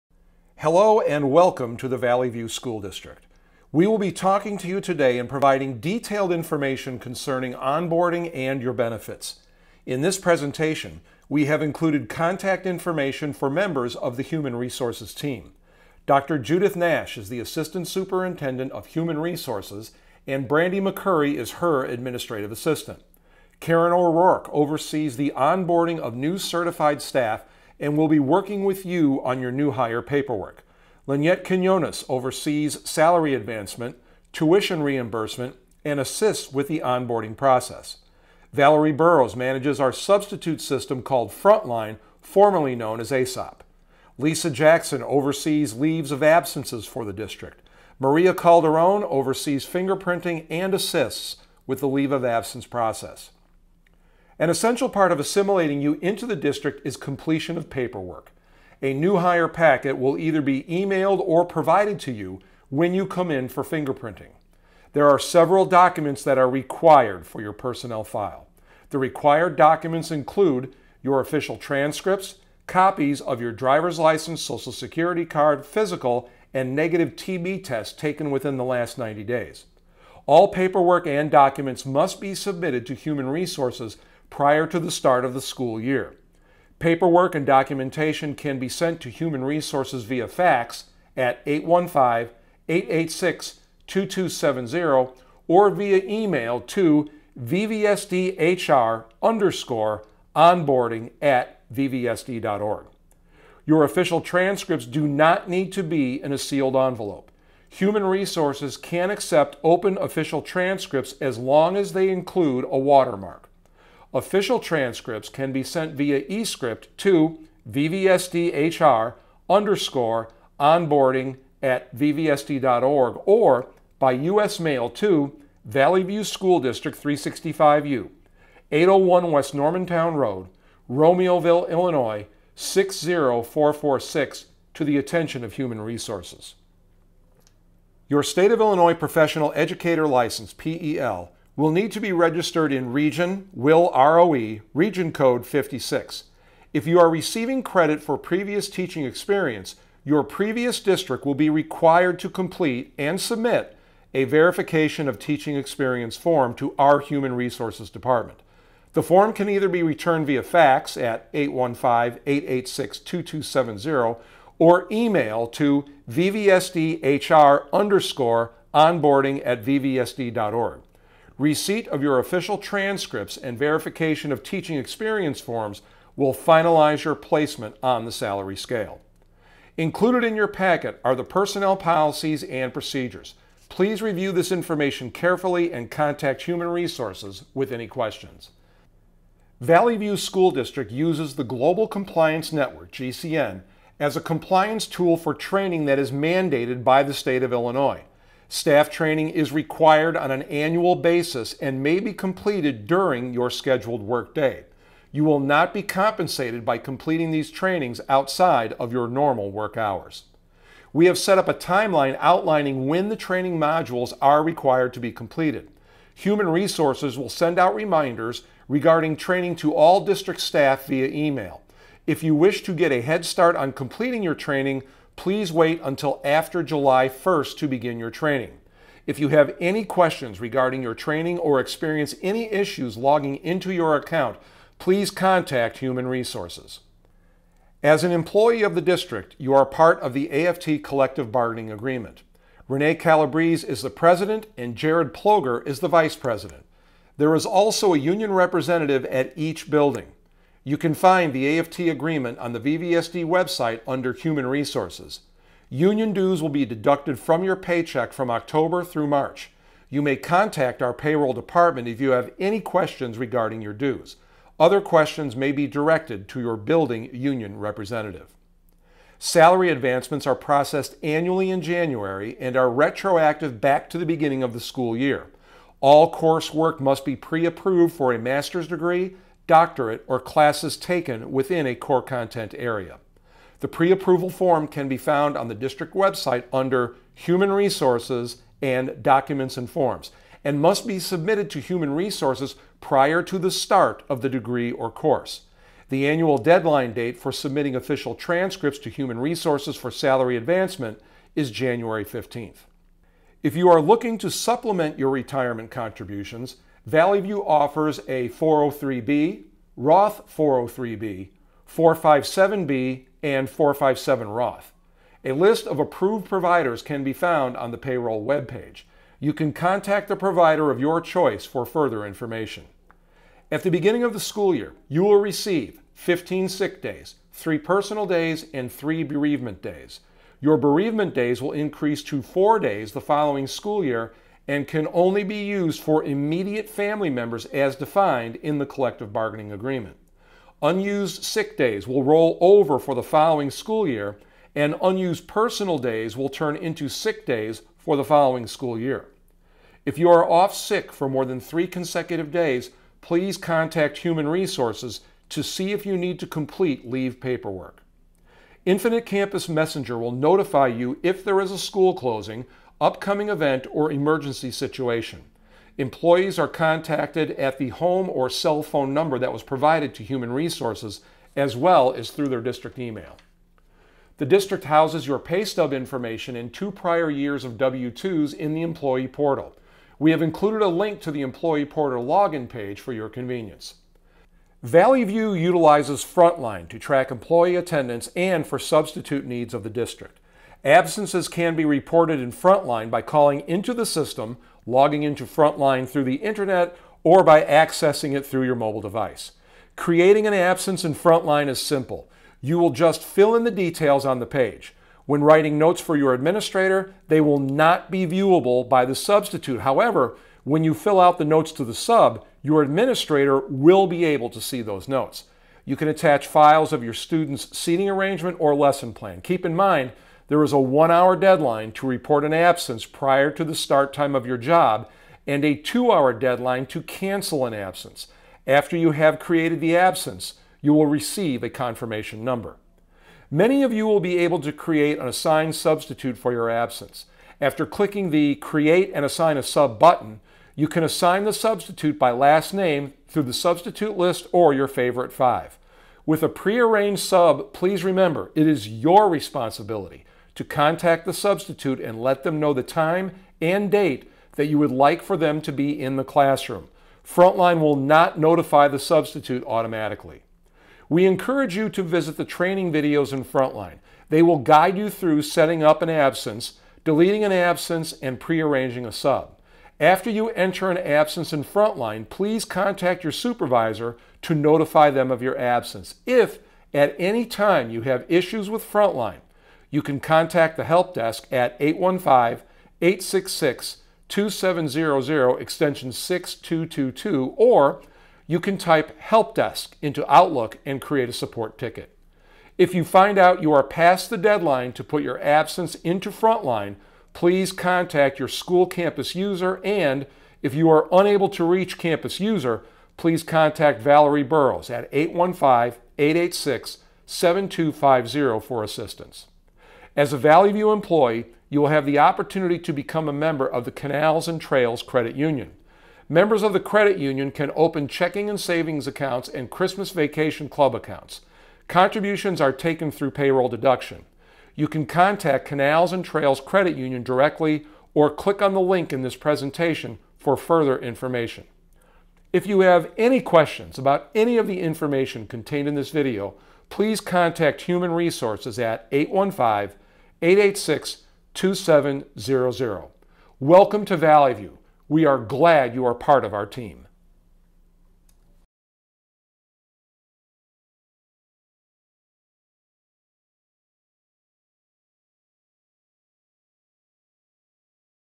VVSD Video for New Team Members Posted April 20 2021
This video provides an overview of Human Resources Department Services for team members joining VVSD.